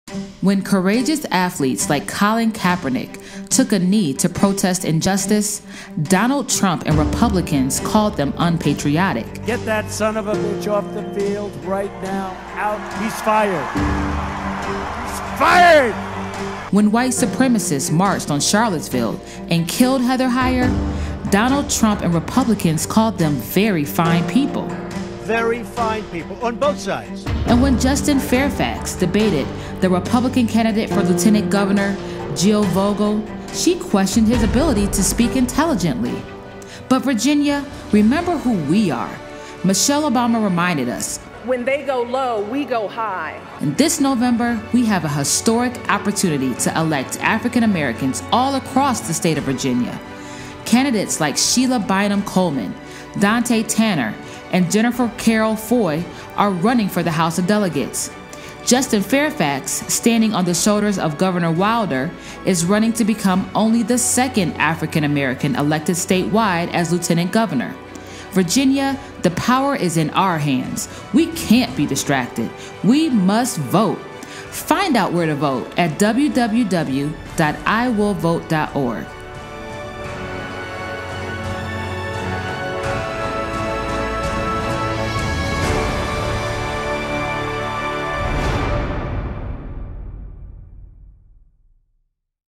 Virginia - We Must Vote
Virginia - despite the resurgence of white supremacy in our politics, remember who we are! We Must Vote!